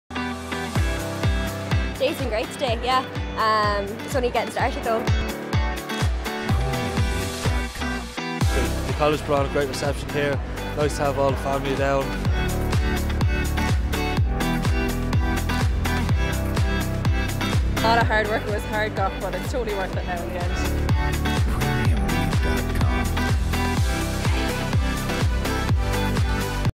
IT Carlow - Conferring 2019
A snippet of Institute of Technology Carlow's Conferring ceremonies at our Carlow campus. Congratulations to all our new graduates.